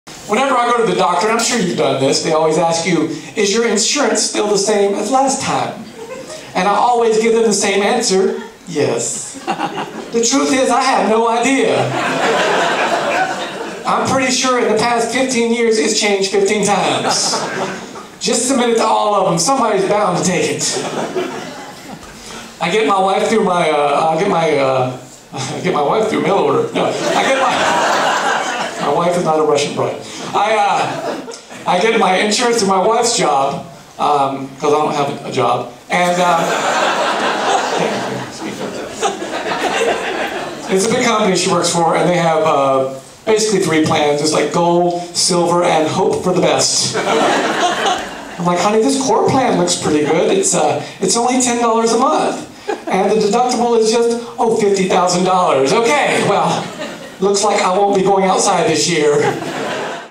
Health Insurance Humor - Comedian Larry Weaver
"I don't have a job... It looks like I won't be going outside this year."
So prophetic!

HEALTH INSURANCE CARD
Whenever I go to the doctor - and I'm sure you've done this…
They always ask you, “Is your insurance still the same as last time?”
And I always give them the same answer, “Yes.”
The truth is I have no idea.
(laughing)
I'm pretty sure in the past 15 years it has changed 15 times.
Just submit it to  all of them. Somebody's bound to take it.
(laughing)

I get my wife through my… I  get my uh… I get my wife through mail order.
(laughing)
My wife is not a Russian Bride.

HEALTH INSURANCE PLANS
I get my insurance through my wife's job because I don't have a “job.”
(laughing)
It’s a big company she works for.
They have basically three plans: it's like, Gold, Silver, and “Hope for the Best.”
I’m like , “Honey, this Core plan looks pretty good.”
”It's only ten dollars a month. And the deductible is just… ”
Oh, $50,000. Okay. Well, it looks like I won't be going outside this year.